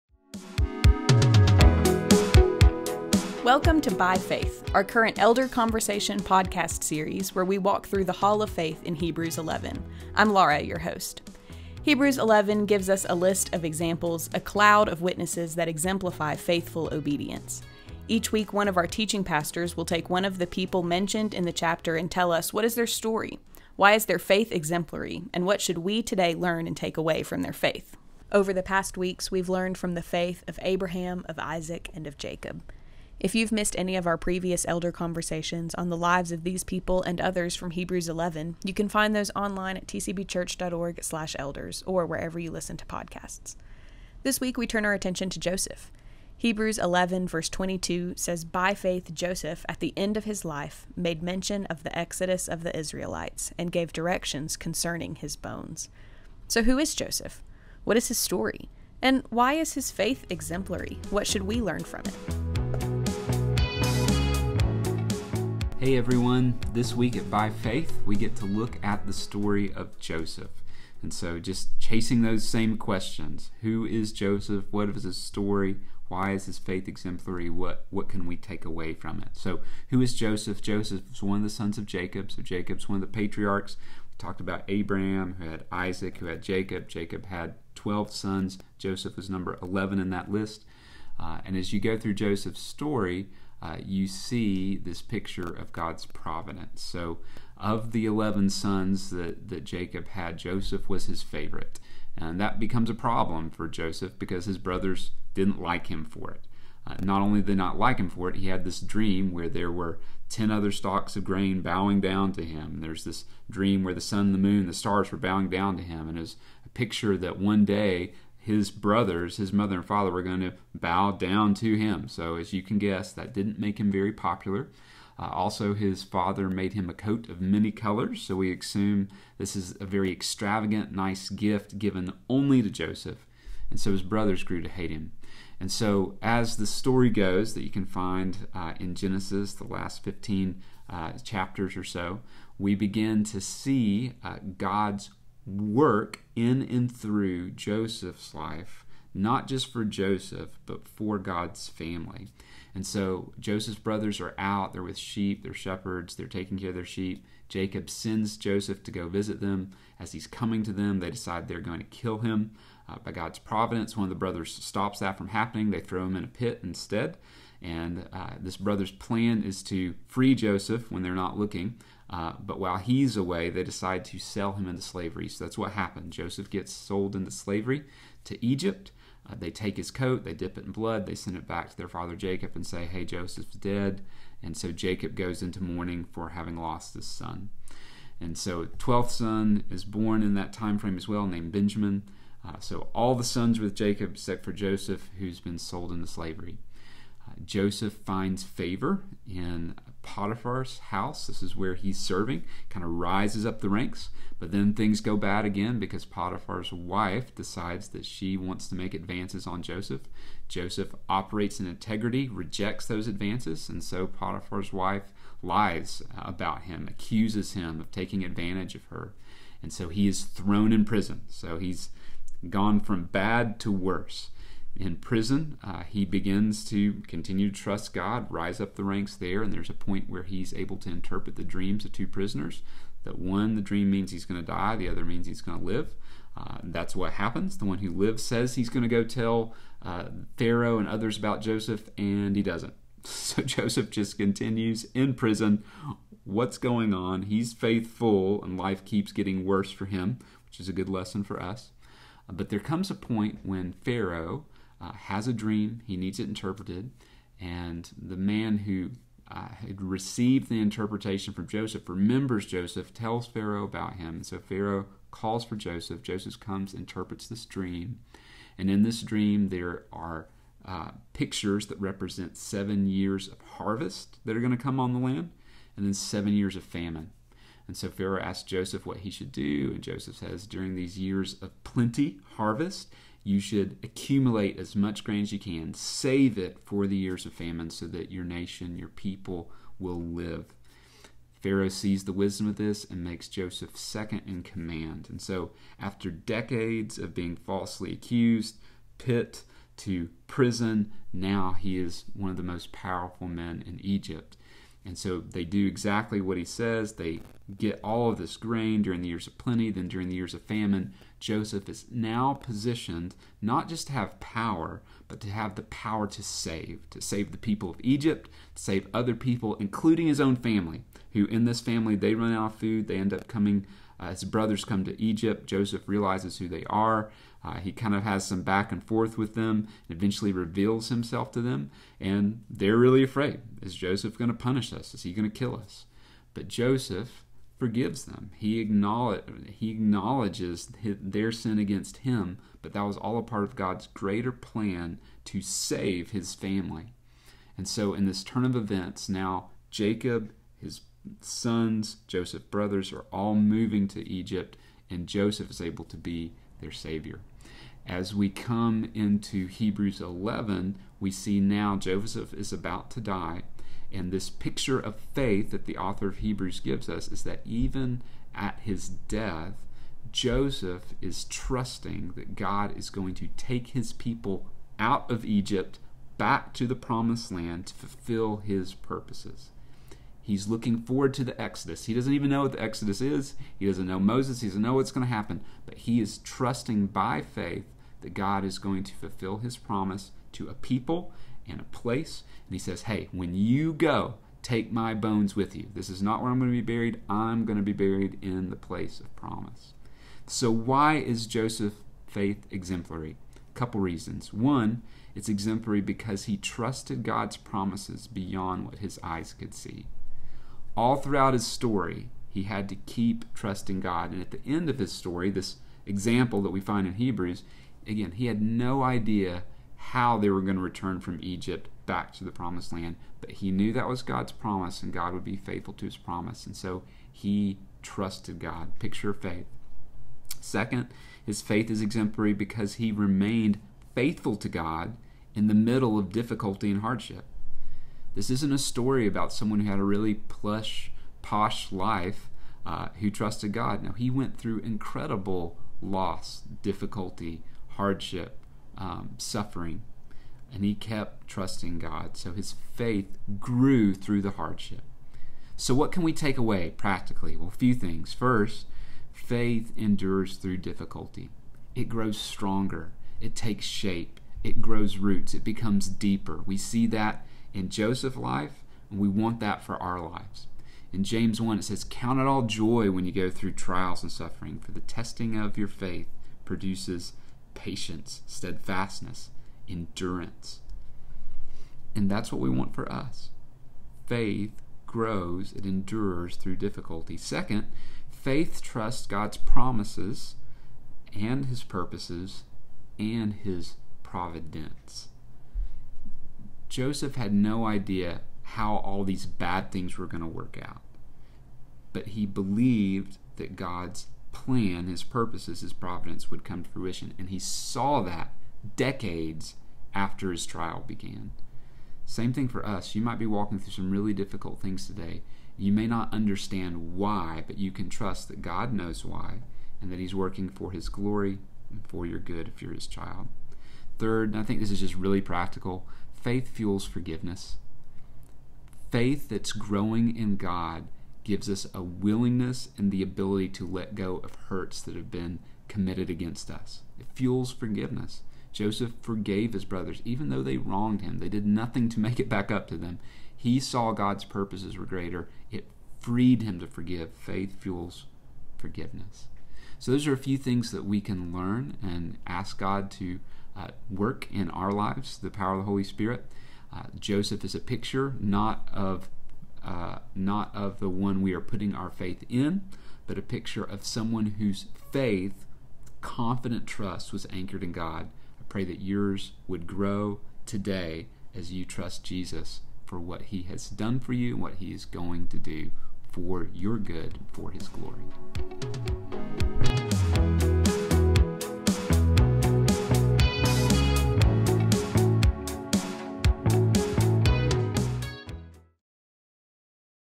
By Faith: Joseph - Episode 9
Message by Paul Mermilliod.

“By Faith” is a supplement to our preaching series in Hebrews that looks specifically at the people mentioned in Hebrews 11 in order to help you pursue practical next steps to live a life of obedient faith that is pleasing to God (Heb 11:6). Each elder conversation will look at one individual: what’s their story? Why is their faith exemplary? What can we, today, learn and take away from their faith?